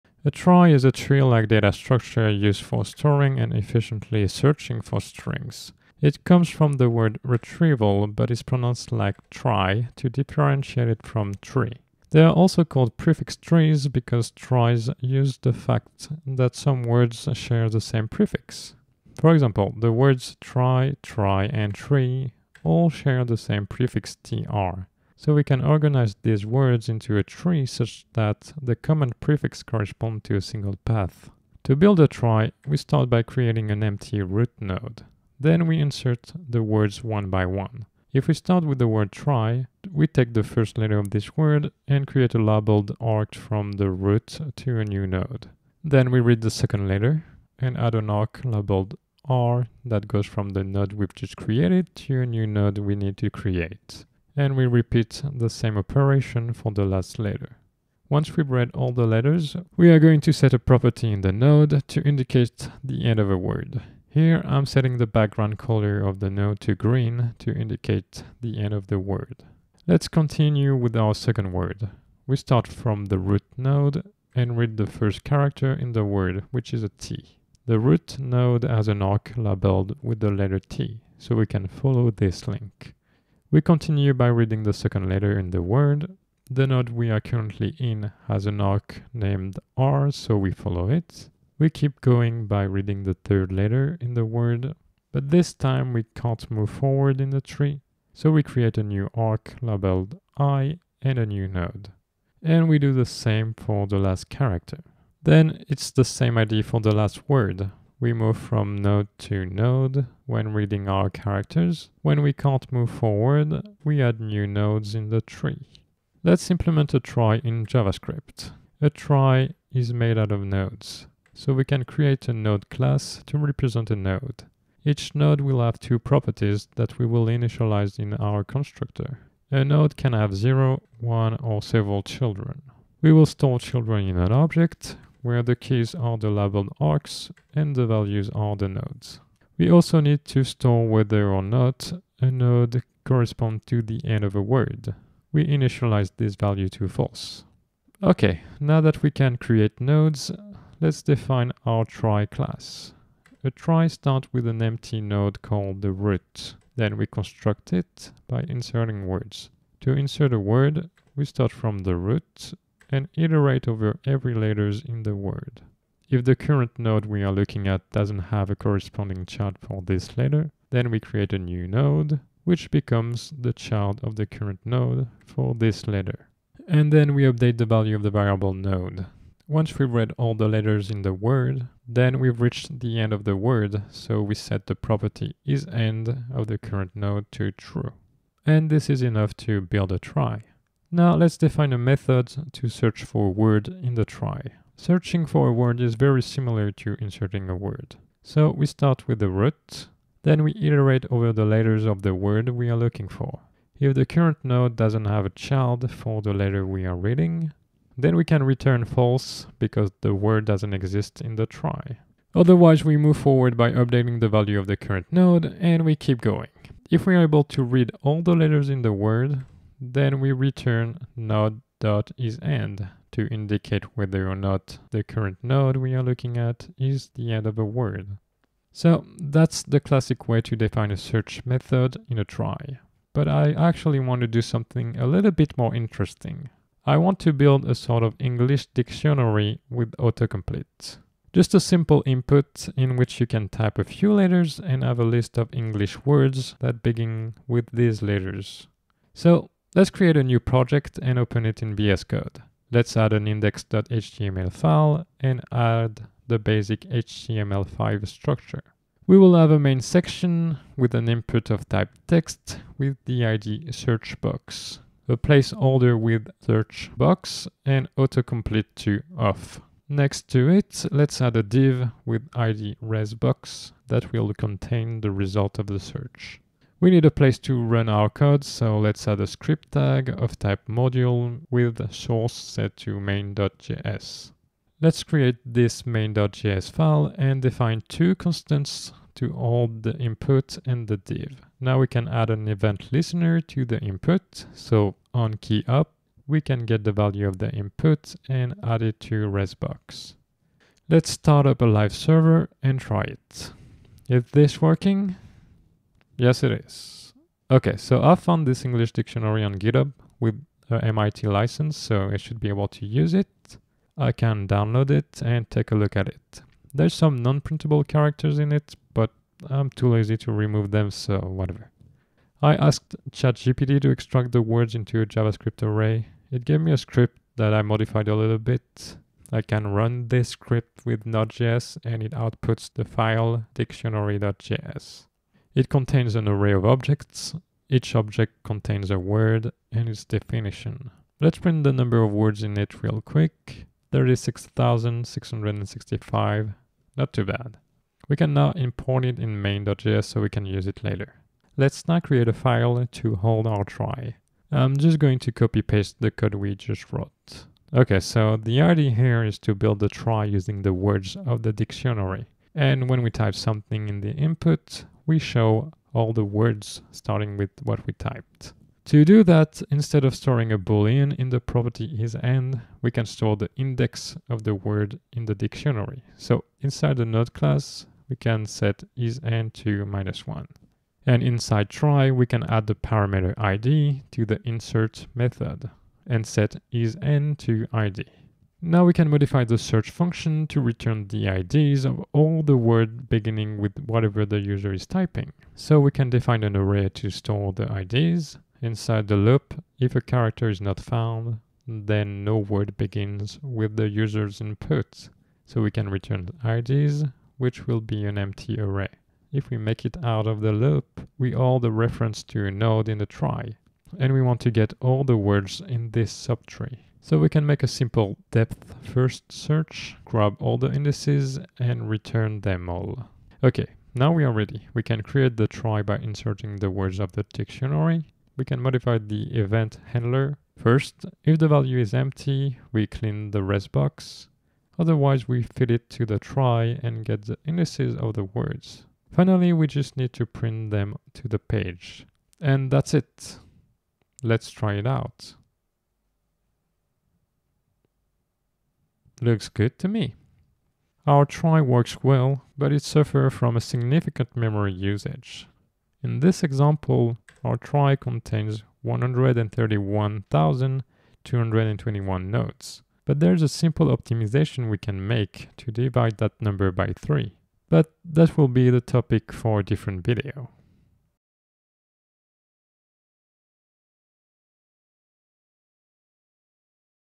Trie - The data structure behind autocomplete (Prefix tree)
Hello, today, we learn about "tries" aka "prefix trees", a simple data structure that can be used to store & search for strings. We implement a simple "autocomplete english dictionary" in JavaScript.

This video can be helpful to solve the medium Leetcode problem "208. Implement Trie (Prefix Tree)"

There are different variations of tries, in this video we implement a very basic one. For more sophisticated ones, check the following videos:

- Patricia Trie: TODO add link?

2nd Channel: @computerbreadboard

Chapters

00:00 Introduction to trie
01:56 Implementation
03:56 Application: autocomplete English dictionary
07:32 Memory usage

*btw sorry for pronouncing "labelled" wrong ^^